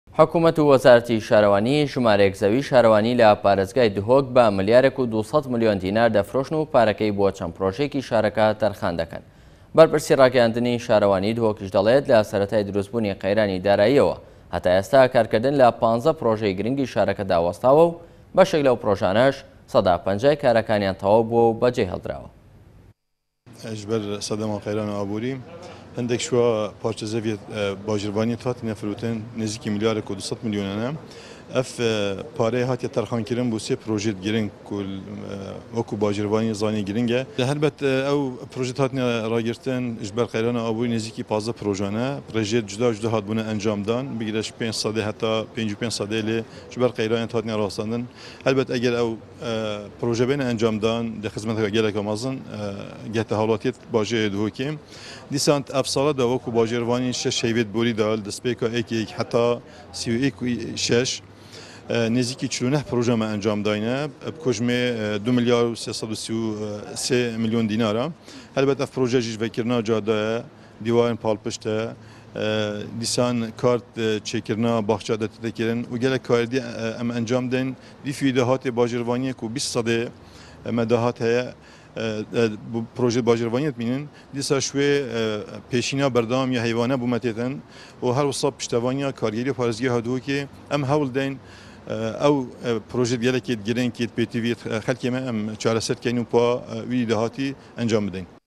شارەوانی دهۆك لە سەرەتای ئەمساڵەوە 25 پارچە زەوی فرۆشتوە
هاوڵاتیانی دهۆك: داهاتی شارەوانیەكان بۆ باخەڵی بەرپرسانە نەك خزمەتی خەڵك

بەڕێوەبەرایەتی شارەوانی دهۆك لە سەرەتای ئەمساڵەوە تا ئێستا 25 پارچە زەوی فرۆشتوە.  هاوڵاتیانی دهۆك ئاماژە بەوەدەكەن,  ئەو داهاتە بۆ خزمەتی خەڵك نەبوە بەڵكو وەك داهاتی نەوت و باج و داهاتەكانی دیكە بۆ باخەڵی بەرپرسان بوە.